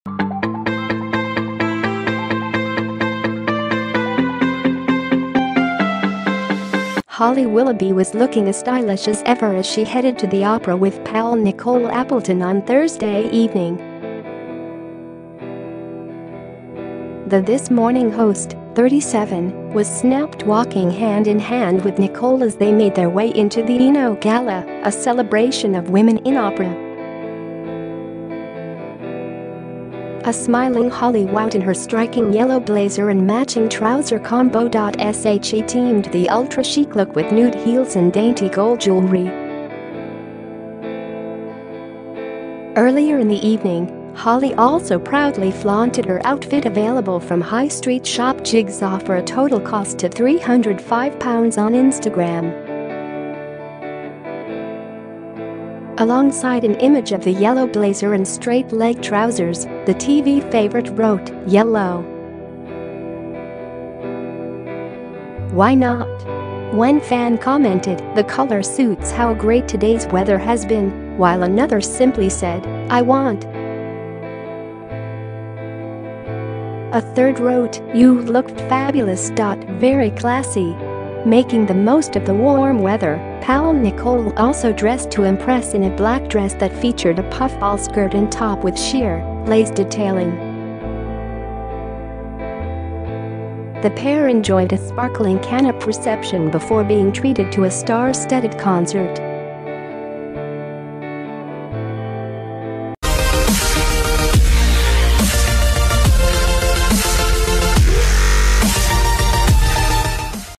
Holly Willoughby shines in chic yellow trouser suit as she holds hands with pal Nicole Appleton - 24
Holly Willoughby was looking as stylish as ever as she headed to the opera with pal Nicole Appleton on Thursday evening.The This Morning host, 37, was snapped walking hand in hand with Nicole as they made their way into the ENO Gala: A Celebration of Women in Opera.A smiling Holly wowed in her striking yellow blazer and matching trouser combo.She teamed the ultra chic look with nude heels and dainty gold jewellery.Earlier in the evening, Holly also proudly flaunted her outfit - available from high street shop Jigsaw for a total cost of £305 - on Instagram.Alongside an image of the yellow blazer and straight leg trousers, the TV favourite wrote: "Yellow.why not." One fan commented: "The colour suits how great todays weather has been," while another simply said, "I want.A third wrote: "You look fabulous.Very classy." Making the most of the warm weather, pal Nicole also dressed to impress in a black dress that featured a puffball skirt and top with sheer, lace detailing.The pair enjoyed a sparkling canapé reception before being treated to a star-studded concert.